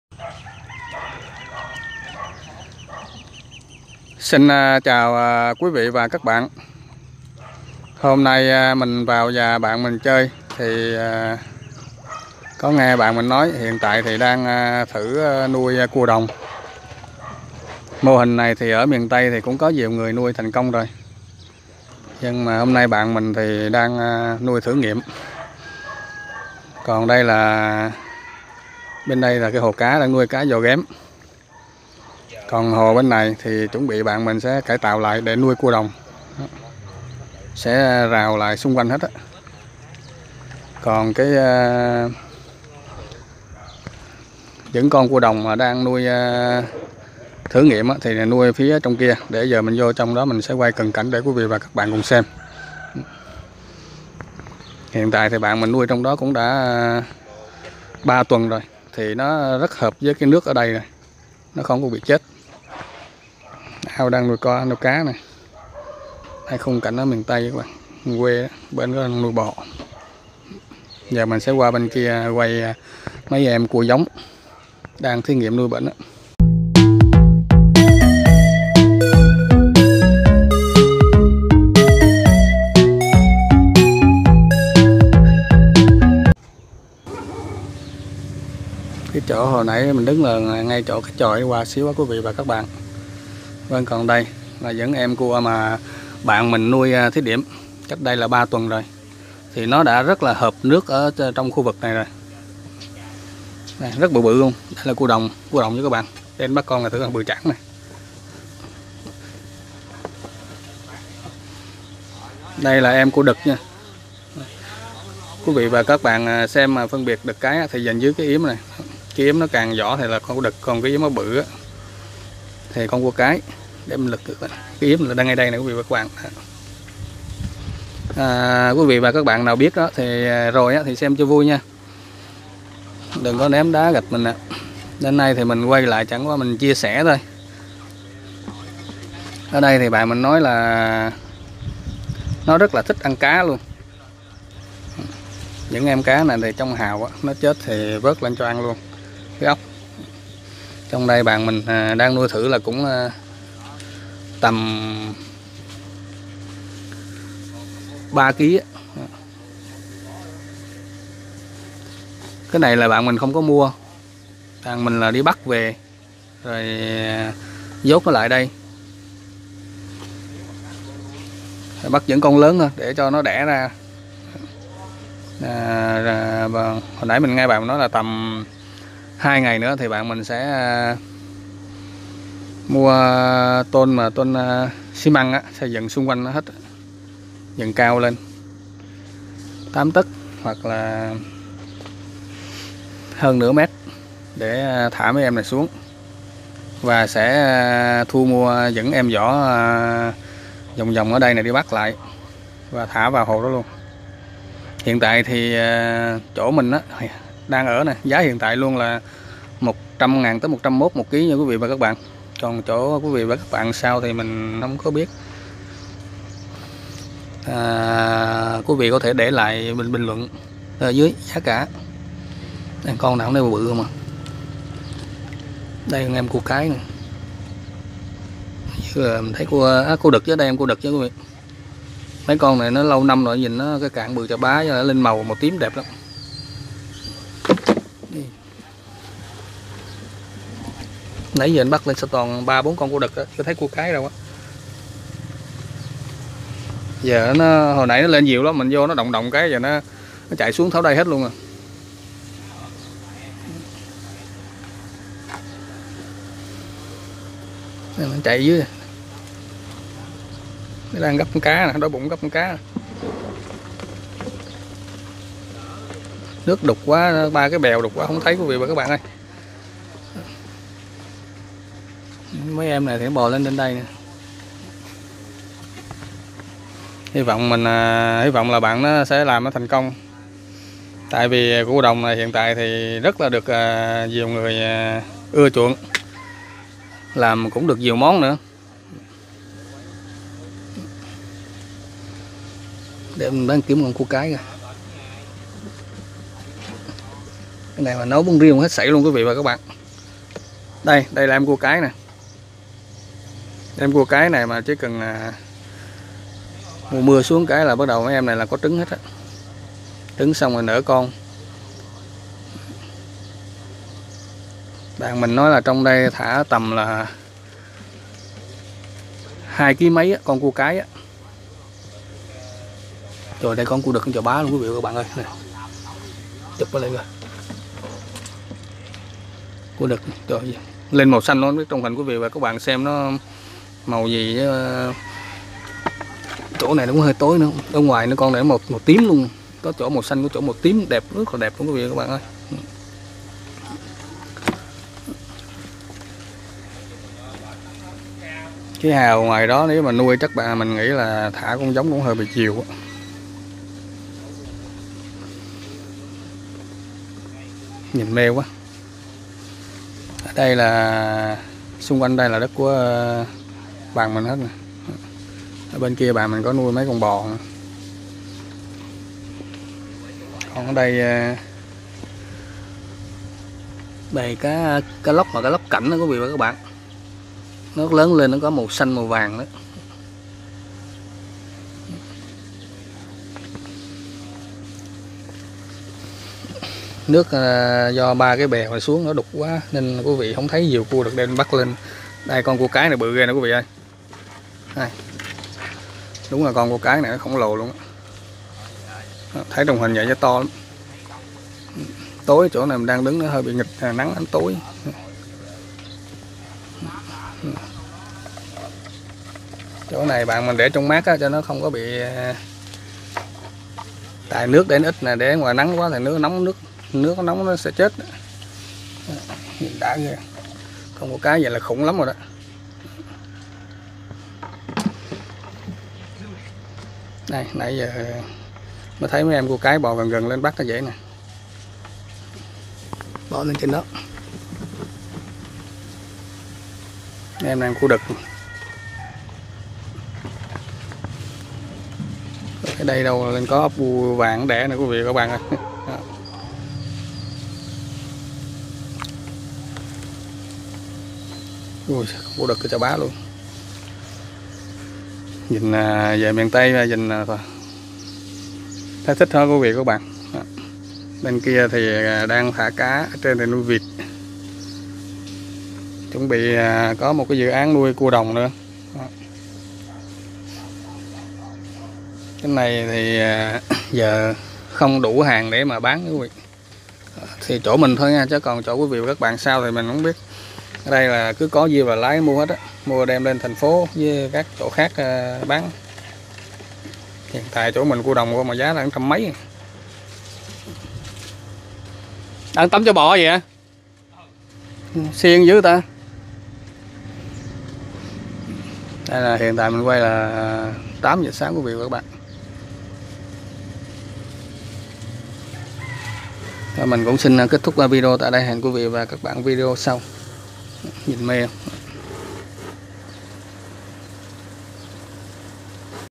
Anh nông dân nuôi thử con cua đồng
Cua đồng giờ được nhiều người thích ăn...